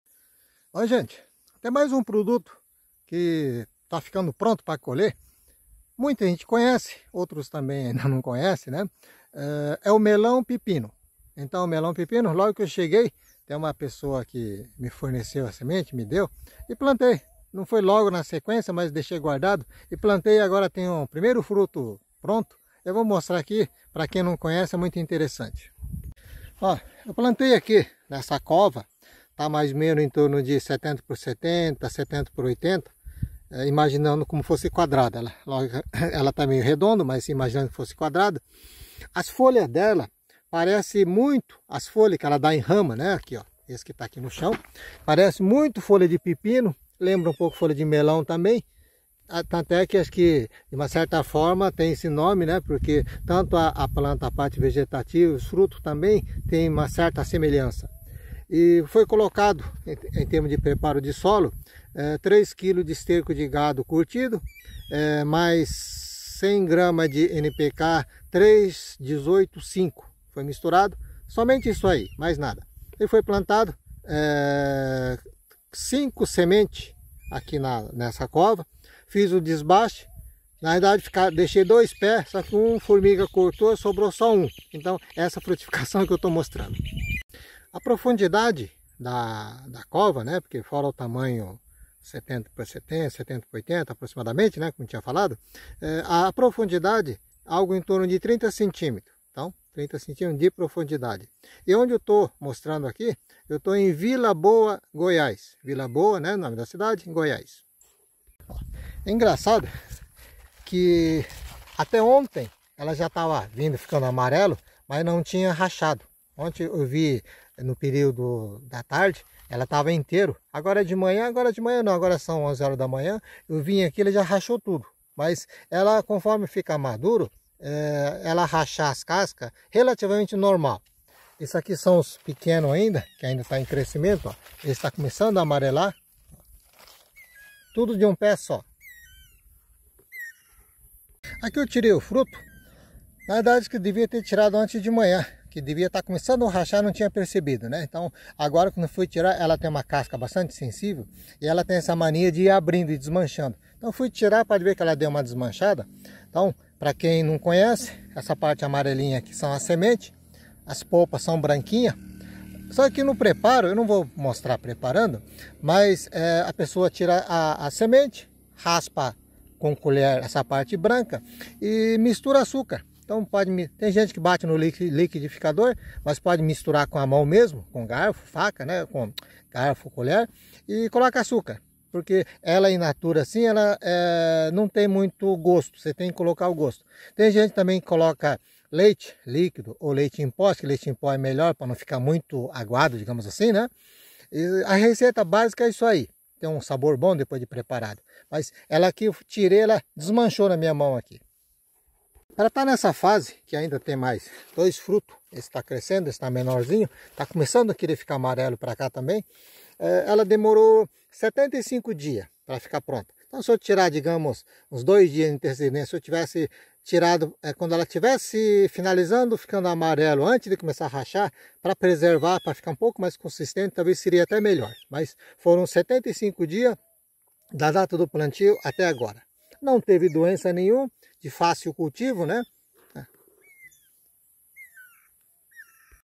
Melão Pepino, uma fruta diferente, não é Melão, nem pepino que conhecemos Tradicionalmente
Ganhei a semente de uma moradora aqui de Vila Boa Goiás.